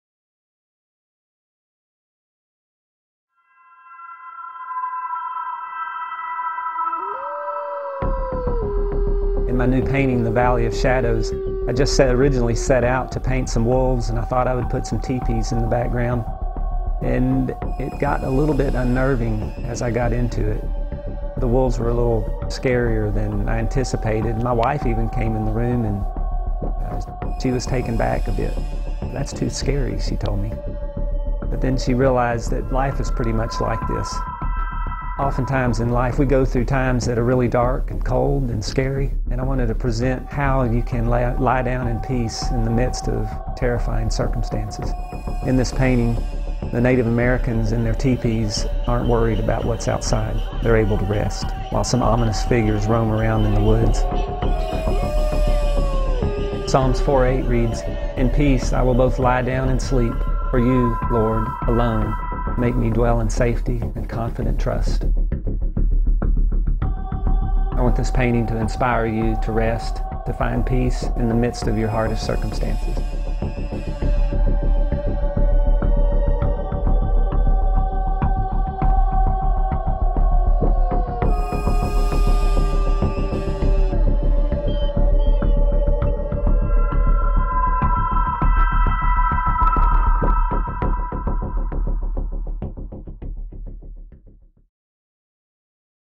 Mark Keathley's Valley Of Shadows
"It is interesting how paintings take on a life of their own. Originally, I simply wanted to create a wolf painting with tepees in the back ground. It wasn't long, however, until the deeper meaning of this painting became clear to me and my wife -- "There is peace and rest to be found in the midst of darkness and danger." Our hope is that you experience this peace even if you, too, find yourself in the Valley of Shadows. Psalm 4:8 reads: "In Peace I will both lie down and sleep, for You, Lord, alone make me dwell in safety and confident trust."

~ Mark and Bonnie Keathley